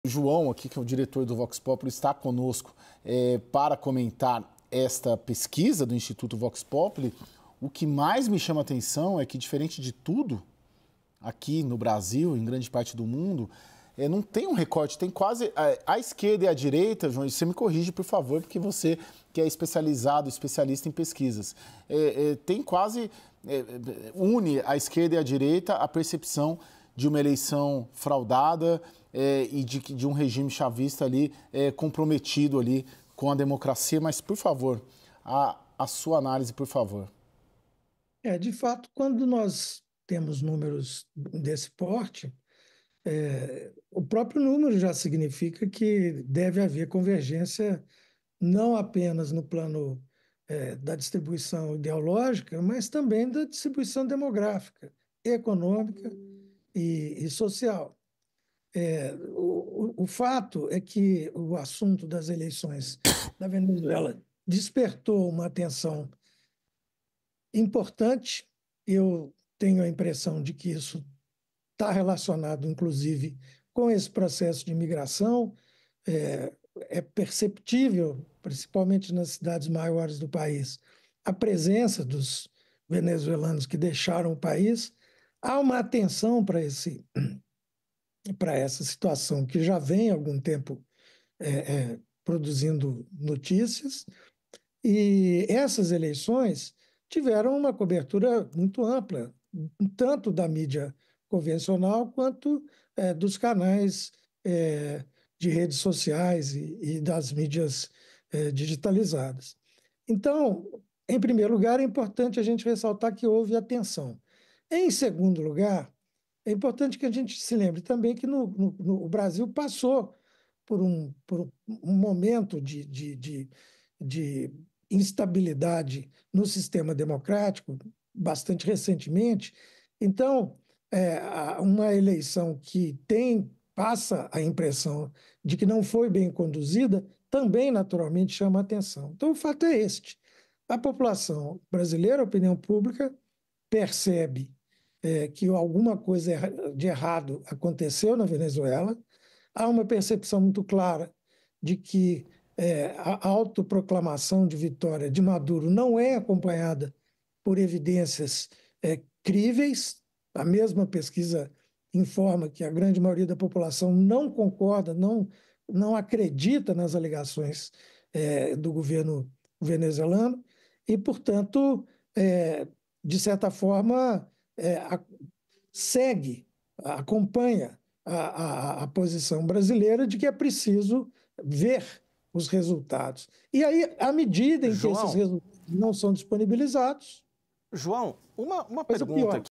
Especialista: Pesquisa informa que maioria da população não acredita no governo venezuelano | WW
Segundo o sócio-fundador do instituto Vox Populli, João Francisco Meira, a pesquisa realizada pela instituição informa que maioria da população não acredita nas alegações do governo da Venezuela e a opinião pública brasileira percebe que alguma coisa de errado aconteceu no país. Além disso, Meira afirma que há uma percepção de que a autoproclamação de Maduro não é acompanhada por evidências críveis.